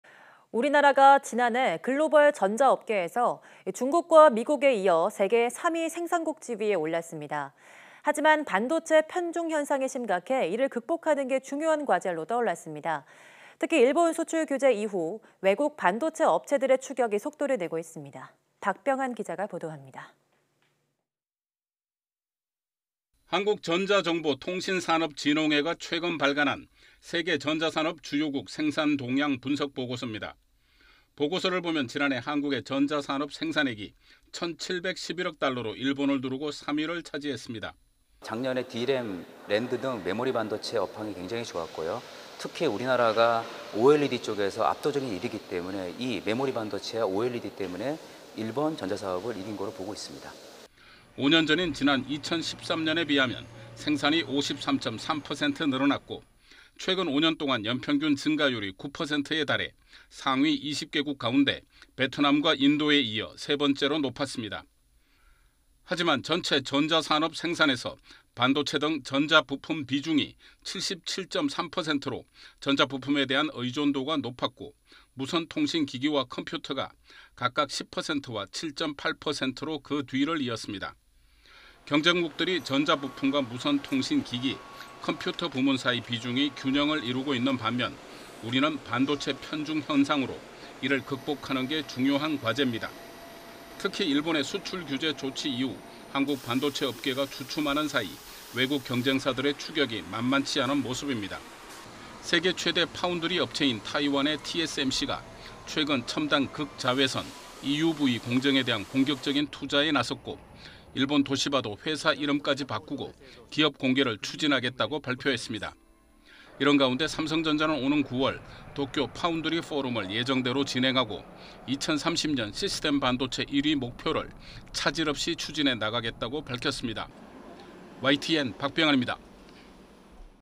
韓 전자산업 생산 日 제치고 세계 3위...반도체 편중 극복 과제 / YTN 사이언스
[앵커]
우리나라가 지난해 글로벌 전자업계에서 중국과 미국에 이어 세계 3위 생산국 지위에 올랐습니다.

하지만 반도체 편중 현상이 심각해 이를 극복하는 게 중요한 과제로 떠올랐습니다.

특히 일본 수출 규제 이후 외국 반도체 업체들의 추격이 속도를 내고 있습니다.

박병한 기자가 보도합니다.

[기자]
한국전자정보통신산업진흥회가 최근 발간한 '세계 전자산업 주요국 생산동향 분석' 보고서입니다.

보고서를 보면 지난해 한국의 전자산업 생산액이 천711억 달러로 일본을 누르고 3위를 차지했습니다.

[소현철 / 신한금융투자 기업분석부 부서장 : 작년에 D램과 낸드 등 메모리 반도체 업황이 매우 좋았고 특히 OLED에서 압도적 1위라 일본 전자산업을 이겼습니다.]

5년 전인 지난 2013년에 비하면 생산이 53.3% 늘어났고, 최근 5년 동안 연평균 증가율이 9%에 달해 상위 20개국 가운데 베트남과 인도에 이어 세 번째로 높았습니다.

하지만 전체 전자산업 생산에서 반도체 등 전자부품의 비중이 77.3%로 전자부품에 대한 의존도가 높았고,  무선통신기기와 컴퓨터가 각각 10%와 7.8%로 그 뒤를 이었습니다.

경쟁국들이 전자부품과 무선통신기기, 컴퓨터 부문 사이 비중이 균형을 이루고 있는 반면, 우리는 반도체 편중 현상으로 이를 극복하는 게 중요한 과제입니다.

특히 일본의 수출 규제 조치 이후 한국 반도체 업계가 주춤하는 사이 외국 경쟁사들의 추격이 만만치 않은 모습입니다.

세계 최대 파운드리 업체인 타이완의 TSMC가 최근 첨단 극자외선 EUV 공정에 대한 공격적인 투자에 나섰고, 일본 도시바도 회사 이름까지 바꾸고 기업 공개를 추진하겠다고 발표했습니다.

이런 가운데 삼성전자는 오는 9월 도쿄 파운드리 포럼을 예정대로 진행하고 2030년 시스템 반도체 1위 목표를 차질 없이 추진해 나가겠다고 밝혔습니다.

YTN 박병한 입니다.